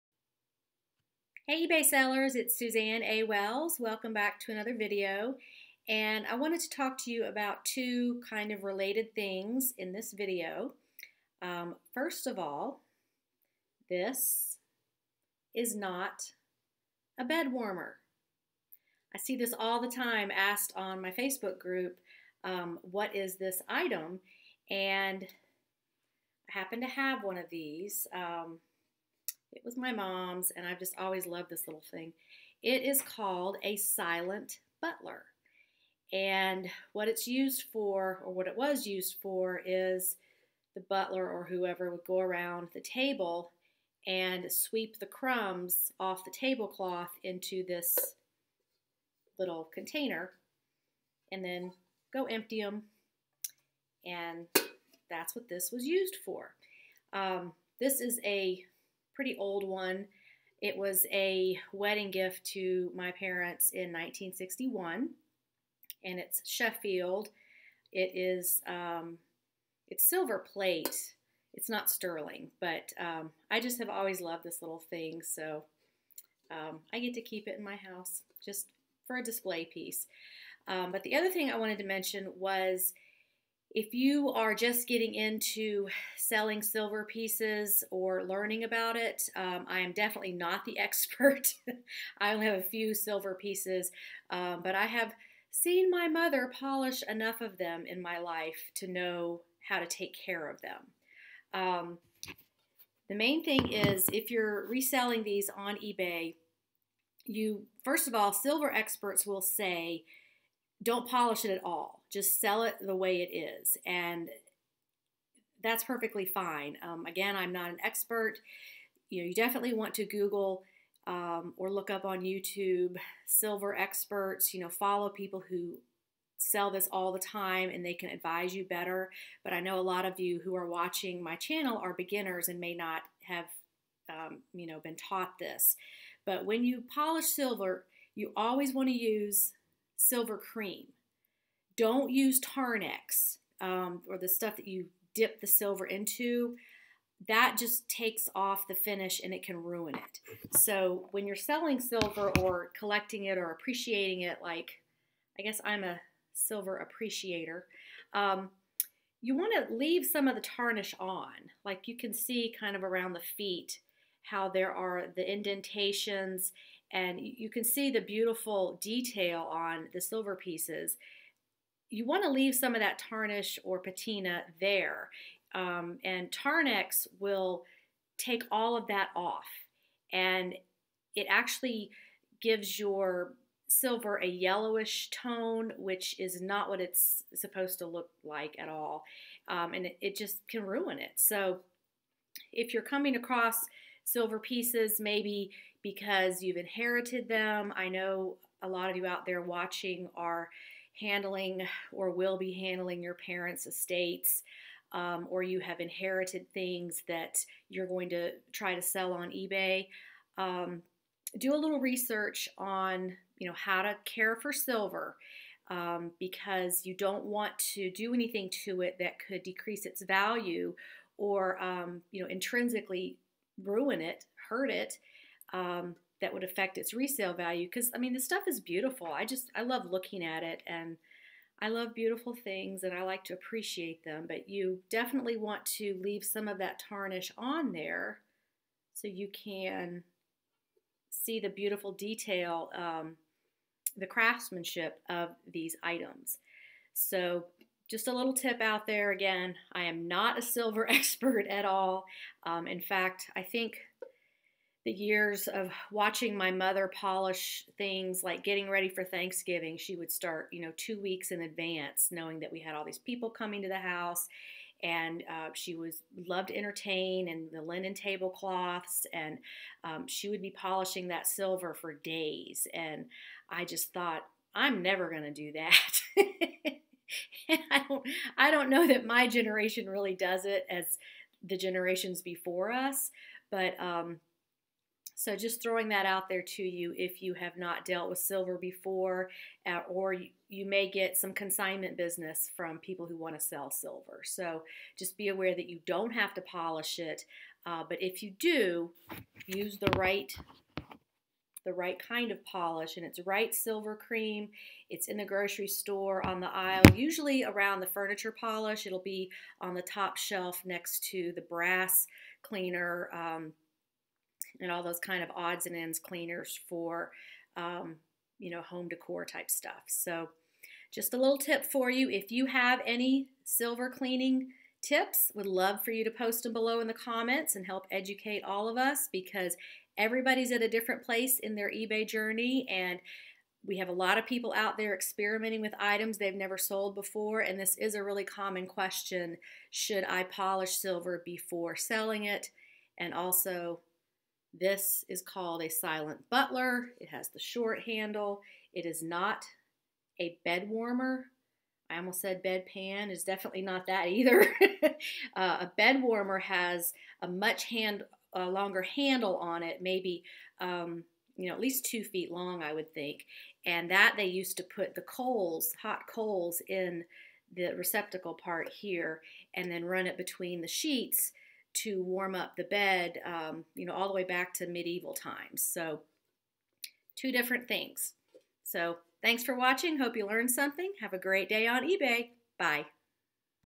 Should You Clean Silver Before Selling on eBay? And a MYSTERY Item!
Join my Premium Content Library for more videos and personal help

This video is a 2 parter! First, the difference between a silent butler and a bed warmer. (Also, silent butlers were used for cleaning ash trays as well as crumbing tables.)

Then, a tip about cleaning silver before selling on eBay - should you? and if you do, the proper way to polish silver.

My eBay Store